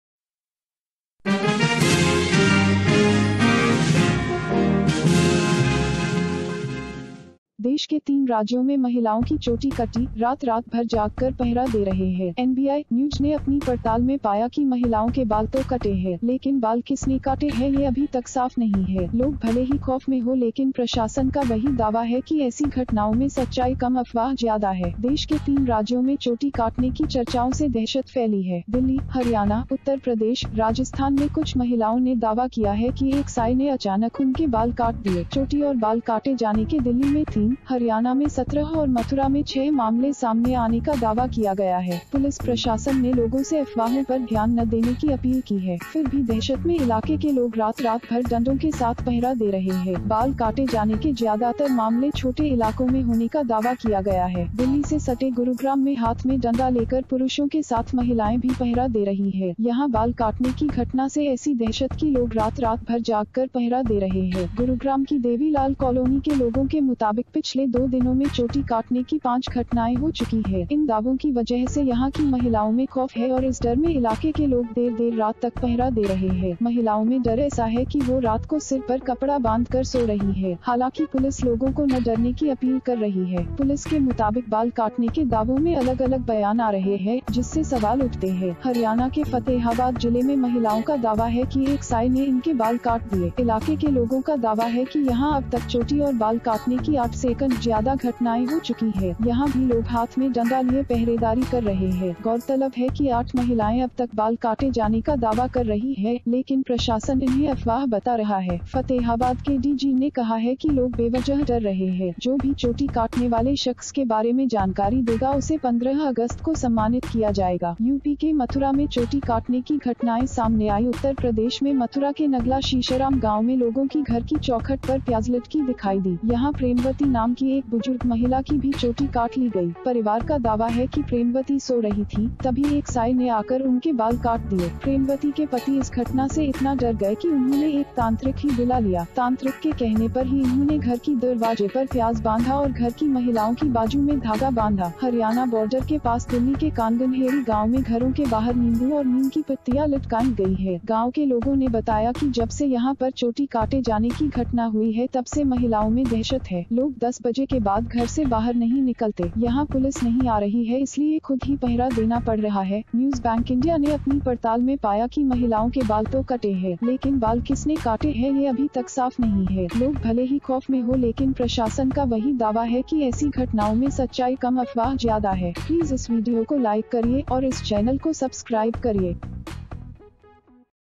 baal katne wali chudail ! baal katne wali chudail ka viral sach
desh ke 4 rajyo me logo ke baal katne wali chudail ! viral sach

देश के चार राज्यों में लोगो के बाल काटने वाली चुड़ैल का वायरल सच .

देश के 3 राज्यों में महिलाओं की चोटी कटी, रात-रात भर जागकर पहरा दे रहे हैं .
N B I ,न्यूज ने अपनी पड़ताल में पाया कि महिलाओं के बाल तो कटे हैं. लेकिन बाल किसनेकाटे हैं ये अभी तक साफ नहीं हैं. लोग भले ही खौफ में हो लेकिन प्रशासन का वही दावा है कि ऐसी घटनाओं में सच्चाई कम अफवाह ज्यादा है..
देश के तीन राज्यों में चोटी काटने की चर्चाओं से दहशत फैली है.
दिल्ली, हरियाणा, उत्तर प्रदेश, राजस्थान में कुछ महिलाओं ने दावा किया है कि एक साए ने अचानक उनके बाल काट दिए.
चोटी और बाल काटे जाने के दिल्ली में तीन, हरियाणा में 17 और मथुरा में 6 मामले सामने आने का दावा किया गया है.
पुलिस-प्रशासन ने लोगों से अफवाहों पर ध्यान न देने की अपील की है.
फिर भी दहशत में इलाके के लोग रात-रात भर डंडों के साथ पहरा दे रहे हैं. बाल काटे जाने के ज्यादातर मामले छोटे इलाकों में होने का दावा किया गया है.

दिल्ली से सटे गुरुग्राम में हाथ में डंडा लेकर पुरुषों के साथ महिलाएं भी पहरा दे रही हैं.
यहां बाल काटने की घटना से ऐसी दहशत की लोग रात-रात भर जागकर पहरा दे रहे हैं.
गुरुग्राम की देवीलाल कॉलोनी के लोगों के मुताबिक पिछले दो दिनों में चोटी काटने की पांच घटनाएं हो चुकी हैं.
इन दावों की वजह से यहां की महिलाओं में खौफ है और इस डर में इलाके के लोग देर-देर रात तक पहरा दे रहे हैं.
महिलाओं में डर ऐसा है कि वो रात को सिर पर कपड़ा बांधकर सो रही हैं. हालांकि पुलिस लोगों को न डरने की अपील कर रही है.
पुलिस के मुताबिक बाल काटने के दावों में अलग-अलग बयान आ रहे हैं, जिससे सवाल उठते हैं.